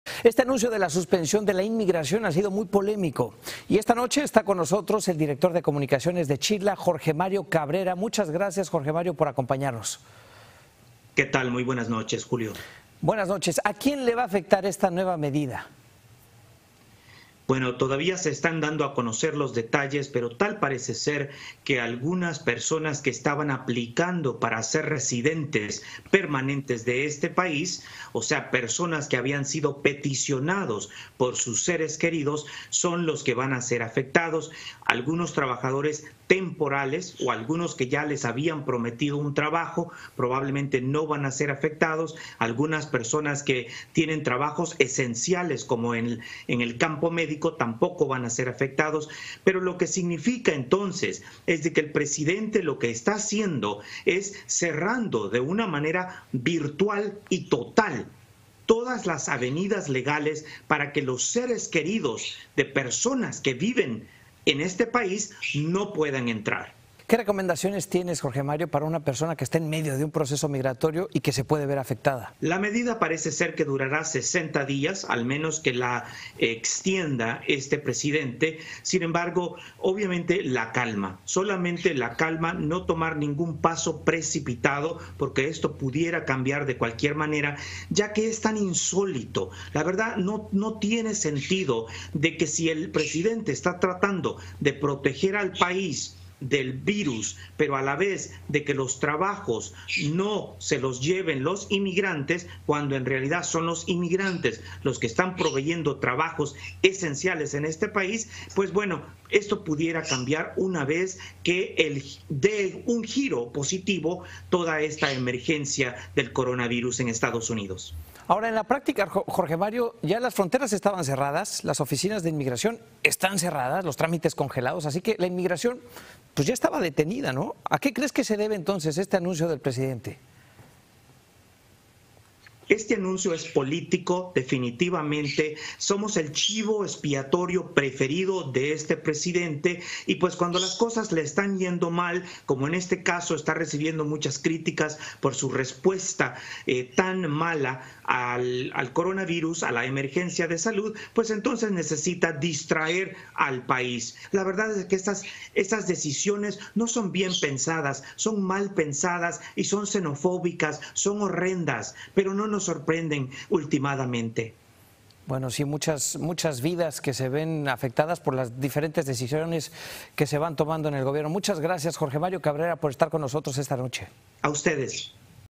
"Personas que habían sido peticionados, serán afectados", afirma Jorge Mario Cabrera | Telemundo
Video oficial de Noticias Telemundo. El Director de Comunicaciones de la Organización Chirla, Jorge Mario Cabrera, afirmó que las personas que se encuentran en medio de un trámite para obtener la residencia, será las más afectadas.